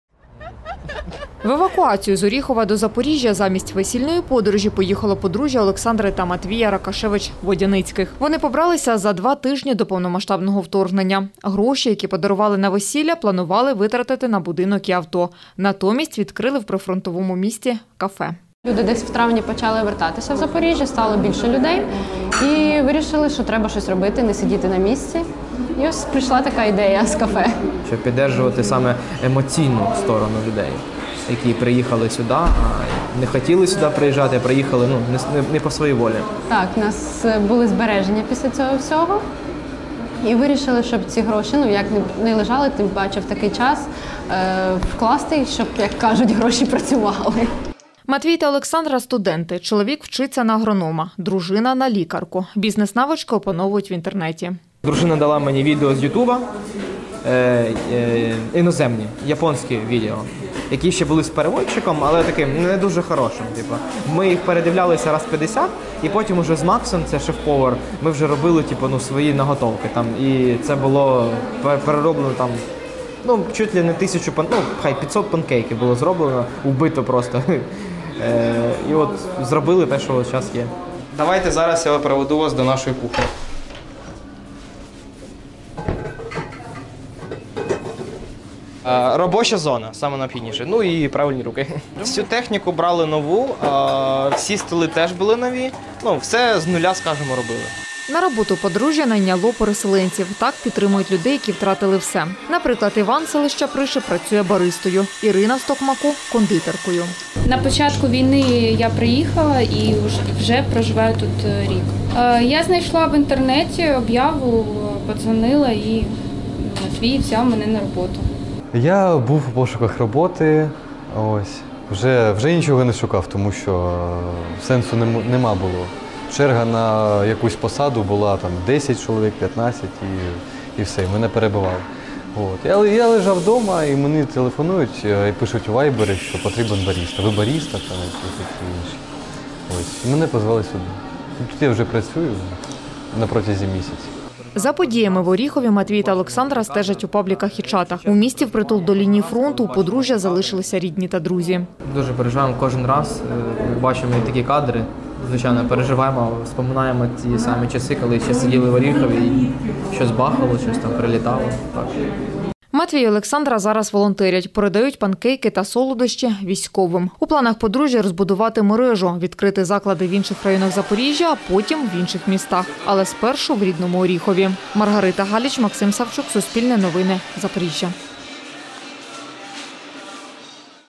Переселенці з Оріхова відкрили кав’ярню у Запоріжжі | Новини
Побралися за два тижні до початку повномасштабної війни, а всі подарункові кошти інвестували у власну справу. Подружжя Матвій та Олександра Ракашевич-Водяницькі переселенці з Оріхова, які торік у березні евакуювалися до Запоріжжя і відкрили кафе. Своєю історією поділилися з журналістами Суспільного.